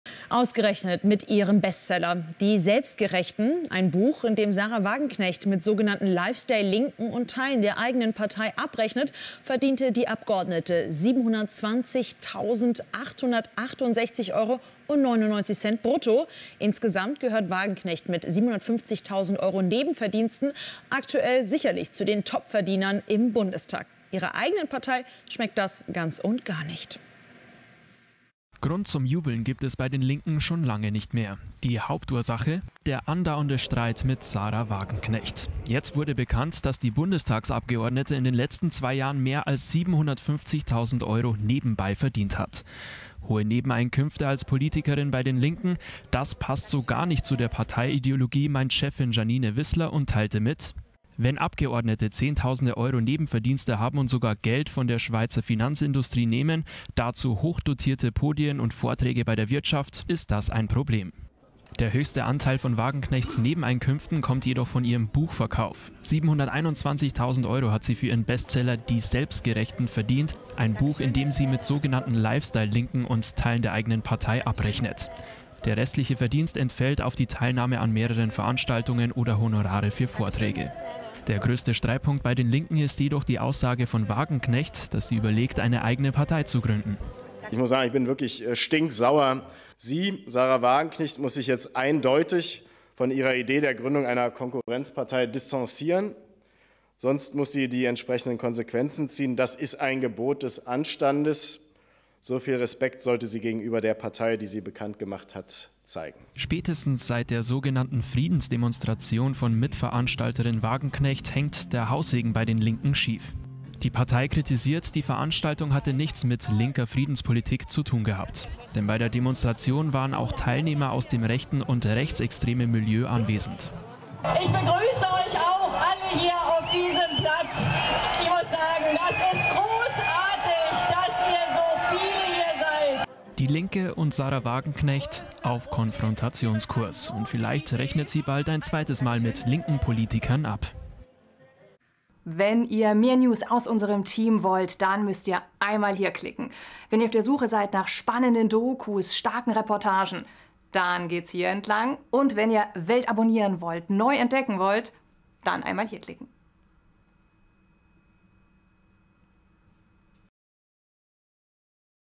STREIT UM SAHRA WAGENKNECHT: 750.000 Euro Honorar! Linken-Chefin Wissler kritisiert Nebeneinkünfte
Linken-Chefin Janine Wissler hat die hohen Nebeneinkünfte der früheren Fraktionsvorsitzenden Sahra Wagenknecht kritisiert. „Wenn Abgeordnete Zehntausende Euro Nebenverdienste haben und sogar Geld von der Schweizer Finanzindustrie nehmen, dazu hoch dotierte Podien und Vorträge bei der Wirtschaft, ist das ein Problem“, sagte Wissler den Zeitungen der Funke-Mediengruppe.

Bundestagsabgeordnete sollten ihre Arbeit im Parlament machen, in Ausschüssen und im Wahlkreis, Linke-Politiker sollten zudem bei Streiks und Protesten präsent sein. „Das muss Priorität haben, das gilt auch für Sahra Wagenknecht.“